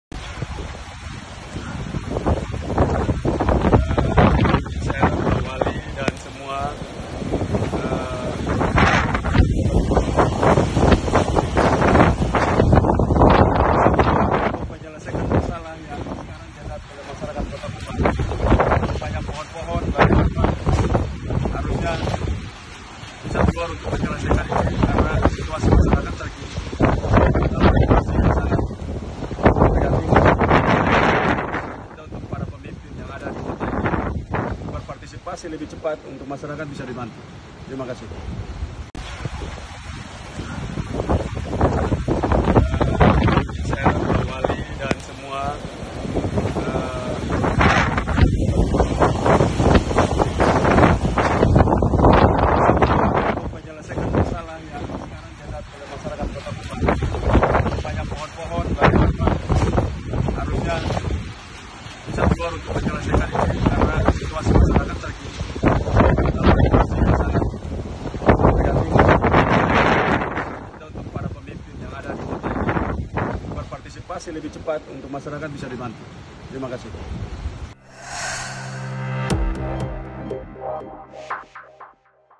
DETIK-detik Gubernur NTT Viktor Bungtilu Laiskodat 'Disambut' Anging Kencang saat Tinjau Rumah Roboh
TRIBUN-MEDAN.COM - Upaya memberikan pertolongan diberikan secepat mungkin kepada warga NTT.

Namun bantuan terhambat karena cuaca ekstrem dan membahayakan.

Akun instagram Partai NasDem memperlihatkan suasana saat Gubernur NTT Viktor Bungtilu Laiskodat meninjau rumah roboh akibat hujan dan angin di Kota Kupang.

Rombongan 'disambut' angin kencang.

Pohon-pohon tumbang disekitar Viktor Bungtilu Laiskodat.

Viktor Bungtilu Laiskodat sempat menengadah kepala melihat sekitarnya hingga berjalan mundur saat angin mulai tak kondusif.